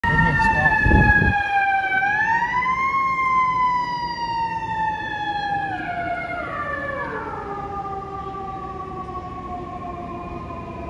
FDNY SQUAD 41 RESPONDING ON BROADWAY IN THE HARLEM AREA OF MANHATTAN IN NEW YORK CITY.
HERE YOU WILL SEE FDNY SQUAD 41 RESPONDING ON BROADWAY IN THE HARLEM AREA OF MANHATTAN IN NEW YORK CITY.